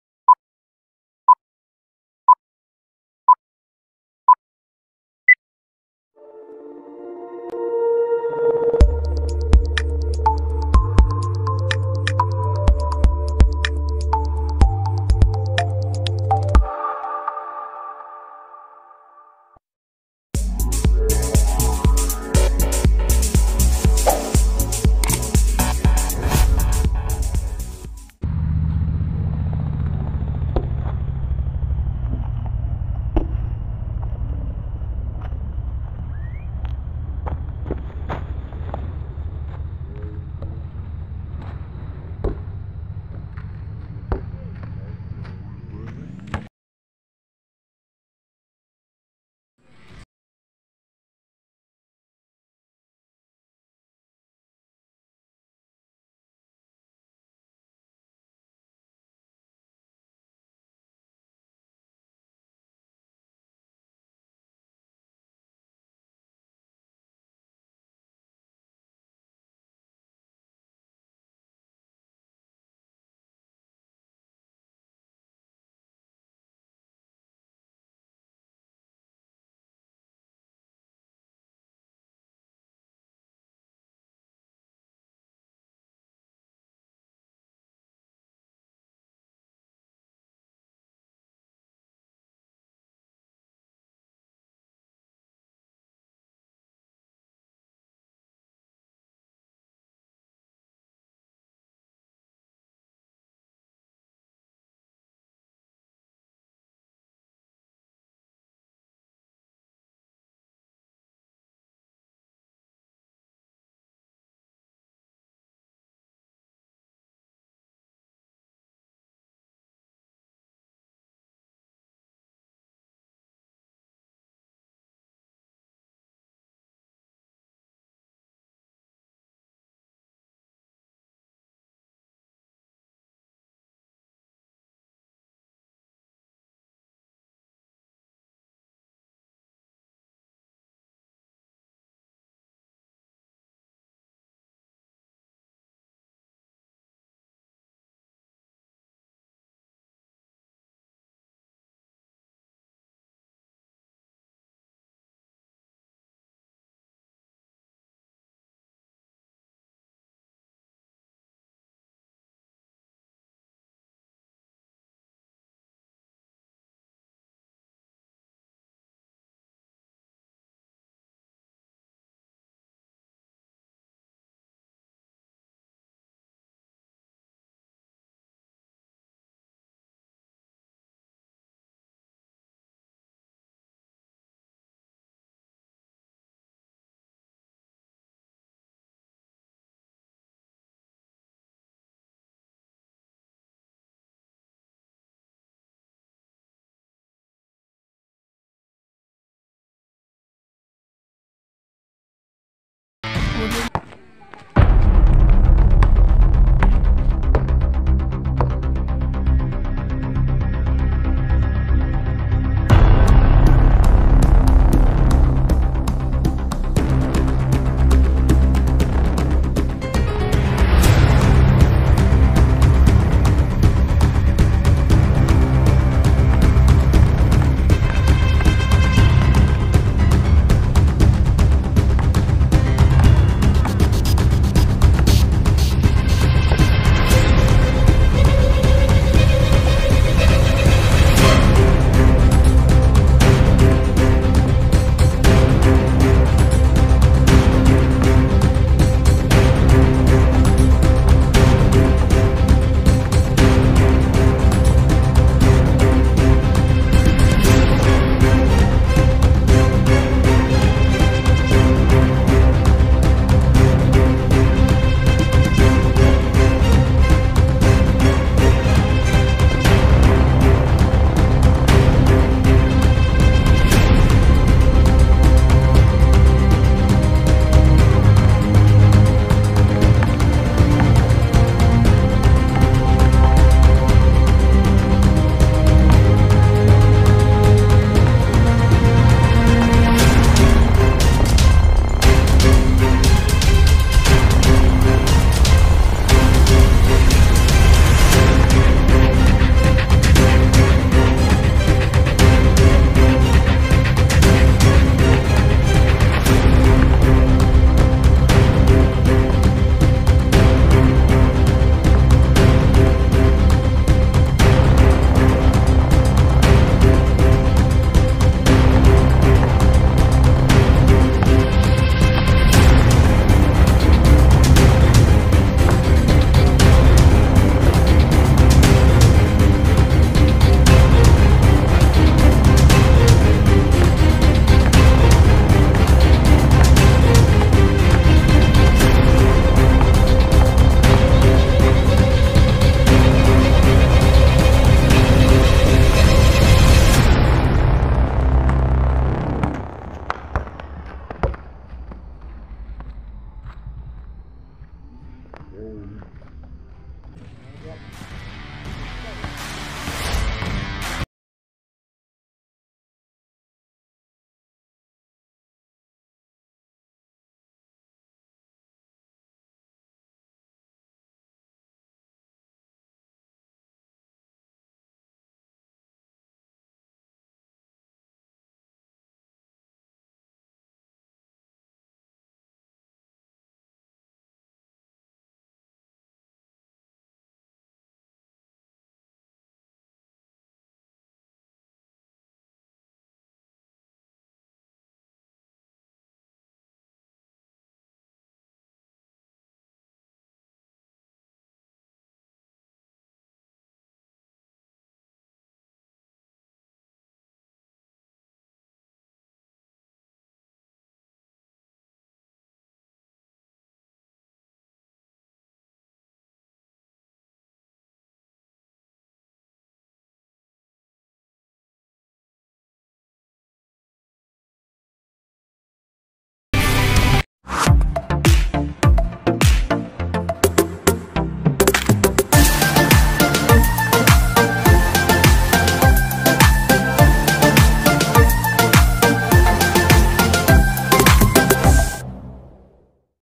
Basketball - Papawis muna kasama ang aking kapatid#1 on 1 basketball
papawis kasama ang aking kapatid
naglaro kami sa court ng School site Brgy. lim-ao kananga Leyte